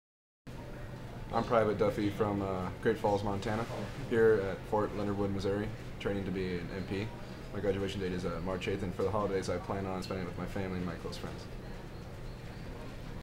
Going home for the holidays
Military personnel based at Fort Leonard wood on their way home for the holidays tells P-D Photographer Robert Cohen what they are looking forward to for the holiday break. Thousand of soldiers flooded Lambert Airport for the annual exodus as they made their connecting flights Wednesday December 21, 2011.